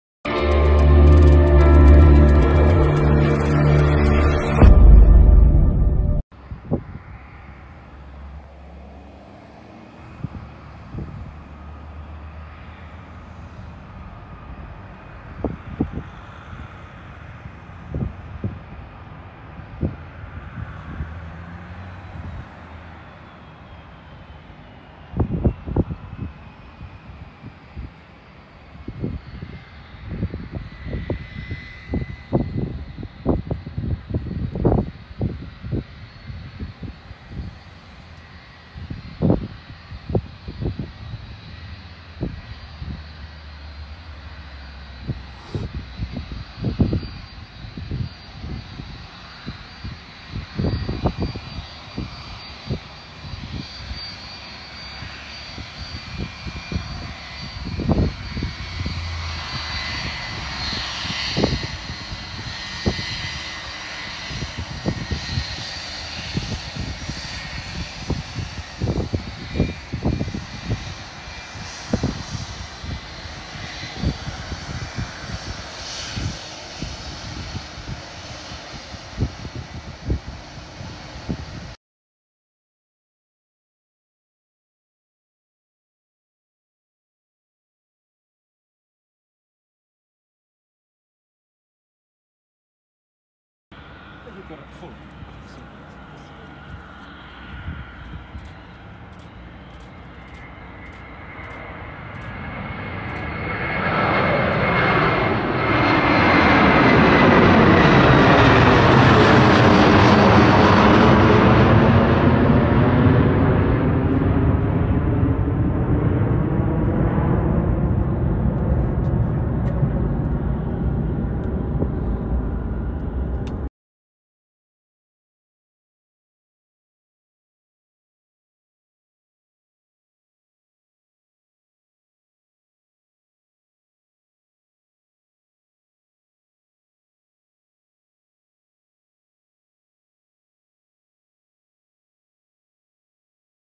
RARE VISITOR - PRIVILEGE STYLE BOEING 757-200 at MADEIRA AIRPORT (FNC || LPMA)
In this video we will show you a Privilege Style Boeing 757-200 visiting at Madeira Airport! Looking for a good way to check some aircraft traffic in Madeira Airport? Then this video is for you as it will show you some airplanes, even the rarest planes, that you can only see at Cristiano Ronaldo Airport!!!

Aircraft seen in the video: Boeing 757-200

Airlines seen in the video: Privilege Style
It would be very aprecciated if you did!
00:00 Intro
00:07 Privilege Style Boeing 757-200 Arriving from Porto
01:22 Privilege Style Boeing 757-200 Pushback
01:34 Privilege Style PVG803P Departure to Porto
02:04 End